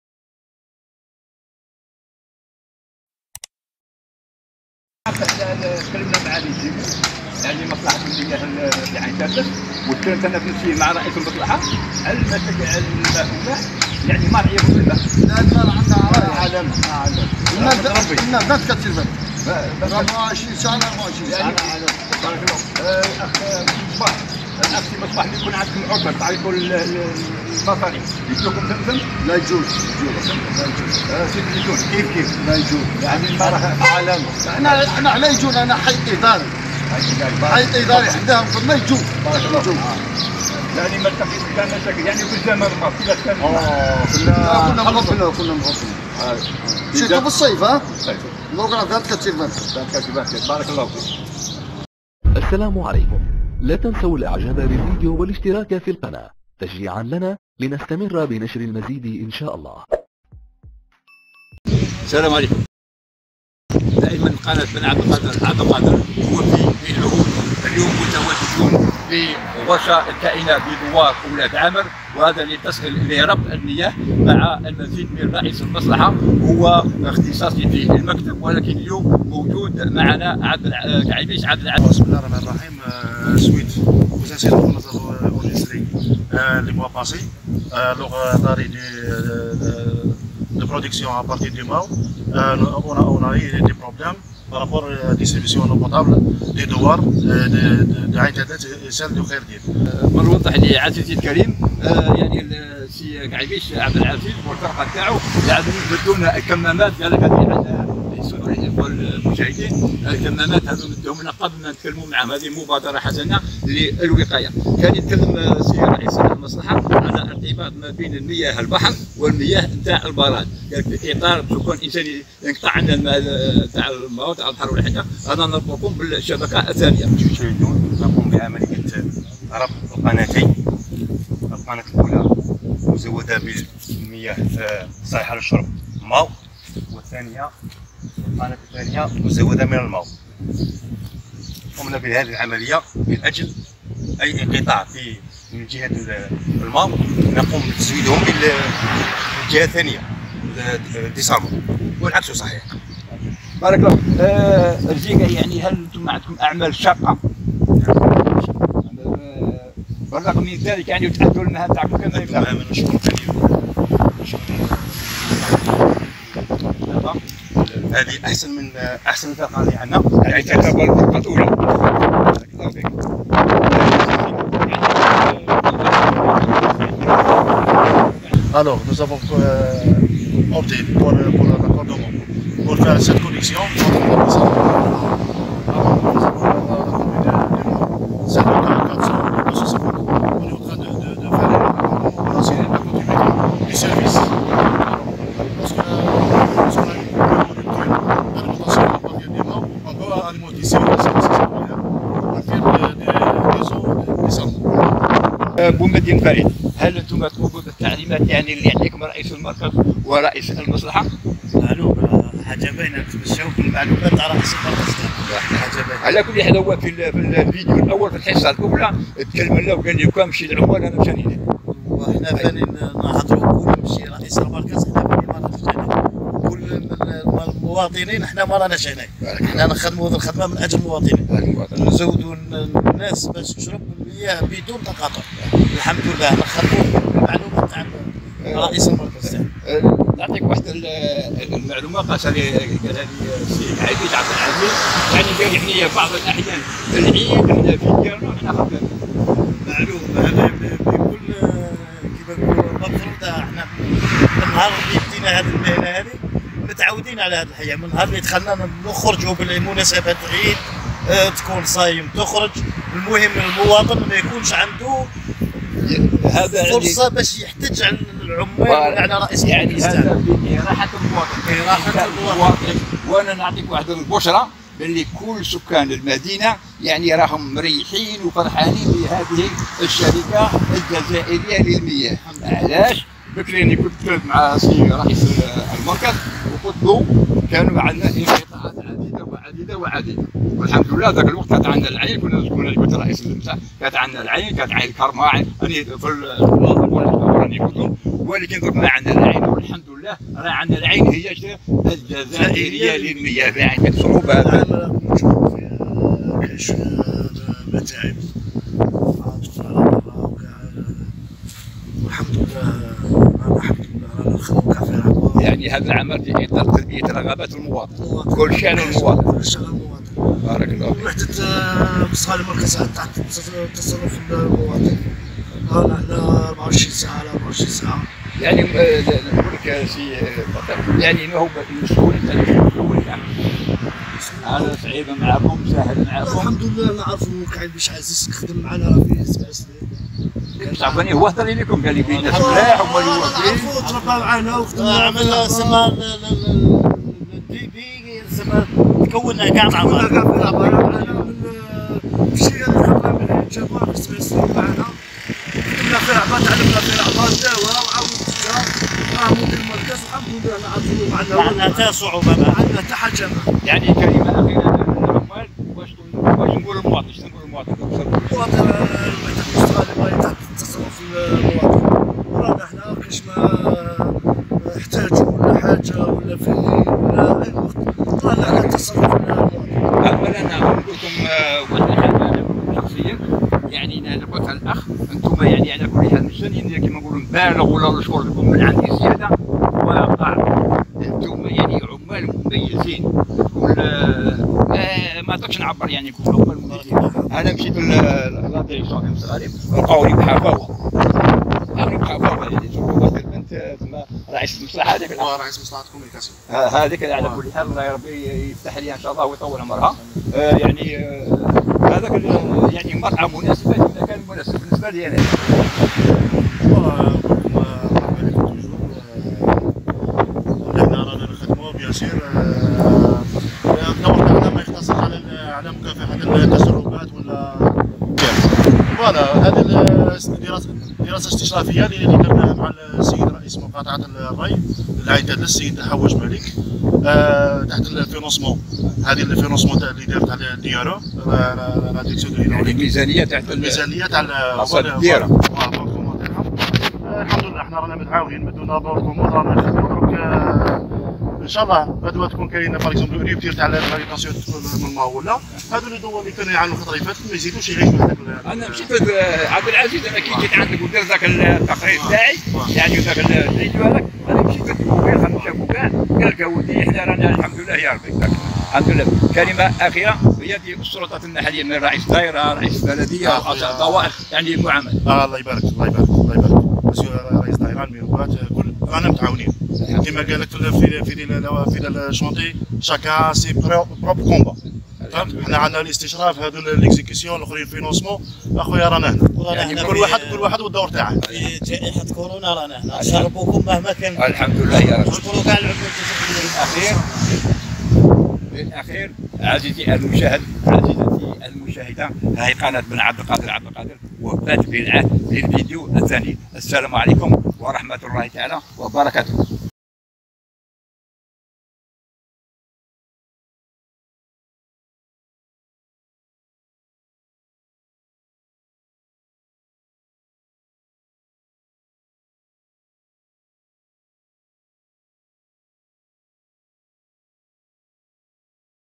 انطباعات المواطنين مع زيارة ميدانية للعمال الجزائرية للمياه عين تادلس رفقة بن كعيبيش رئيس المصلحة
لاتنسى الاشتراك و تفعيل زر الجرس ليصلك جديدنا